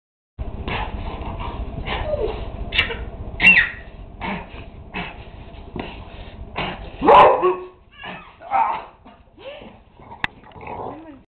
Rommel defendiendo a Christian... Jaral del Progreso.
Mi perro boxer de nombre Rommel defiende a mi hijo de las supuestas nalgadas que le doy.